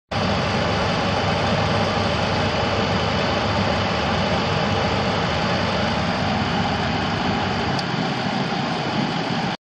2011 Ford Econoline E450 shuttle bus for sale at auction | bidding closes July 9, 2019
2011 Ford Econoline E450 shuttle bus
218,949 miles on odometer
6.8L V10 SOHC 20V gas engine
Automatic transmission
Single axle
AC and heat
Auxiliary controls
Electric passenger sliding door
16 passenger capacity
Wheelchair lift
Spring suspension
Hydraulic brakes
158" wheelbase
225/75R16 tires
GVWR 14,500 lbs
Non-operational wheelchair lift
Kansas title
1FDFE4FS3BDA63161